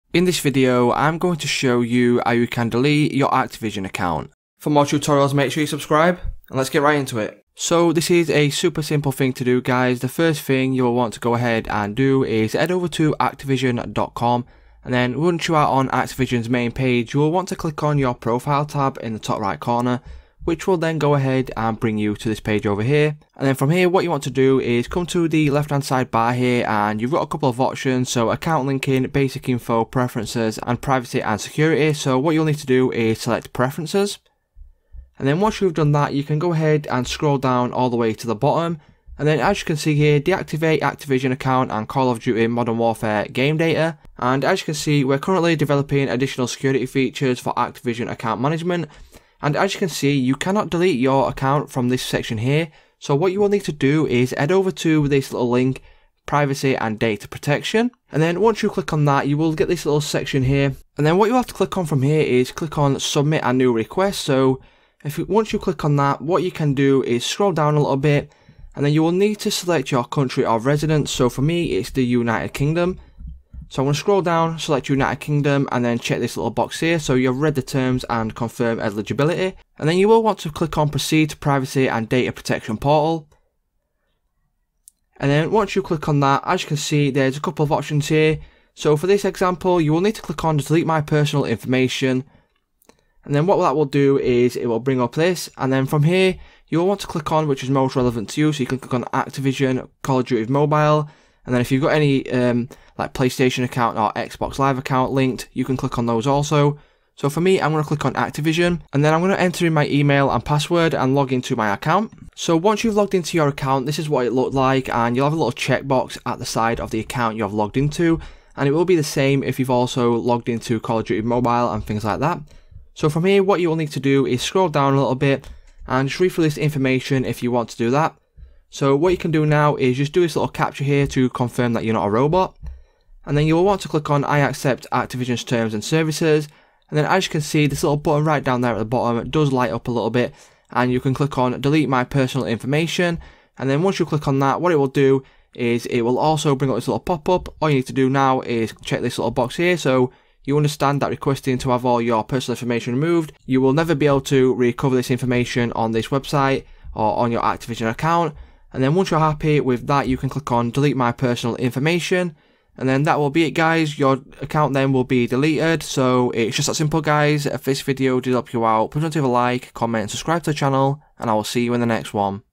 How to Permanently Delete your Activision Account (2026)
In this video, I show you How to Permanently Delete your Activision Account | Warzone | 2026

Steps to delete Activision account:

1) Go to Activision.com
2) Login to your account
3) Select Profile
4) Click Preferences tab
5) Click on Privacy and Data Protection tab
6) Follow steps